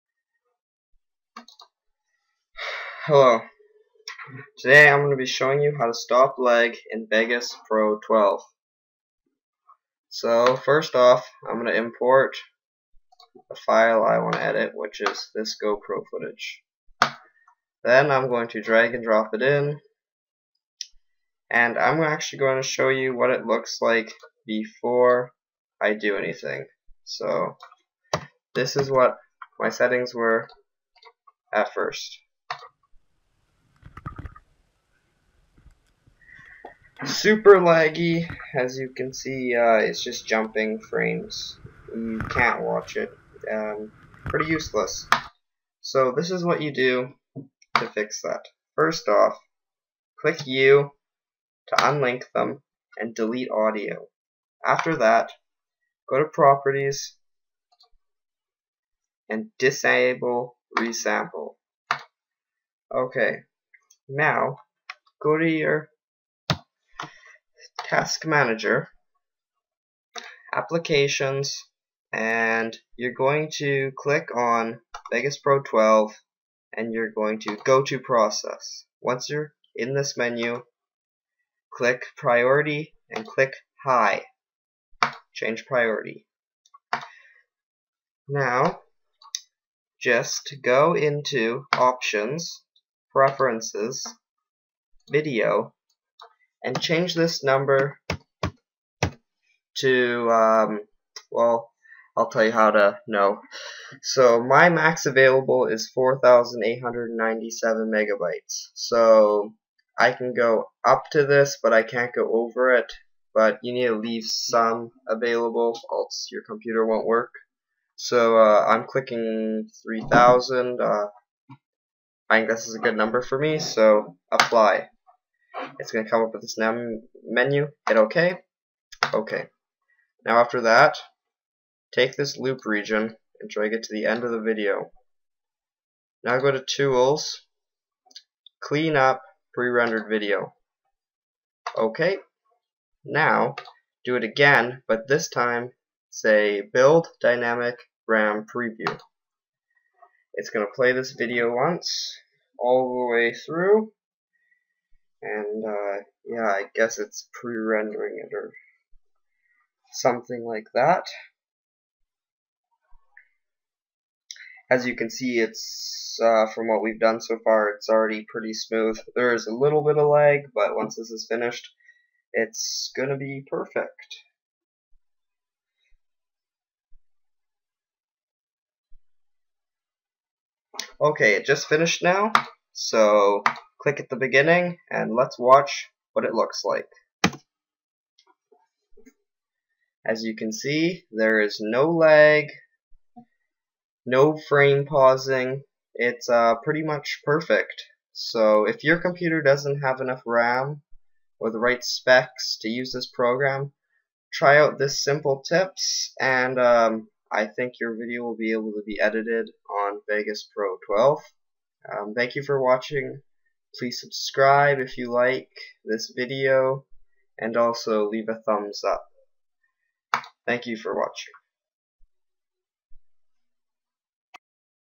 How To Stop Lag In Sony Vegas Pro
In this video I will show you how to eliminate lag in the preview
This works flawlessly
This is my method I made up and have not seen in on any other sites before
And I hope this fixes you issues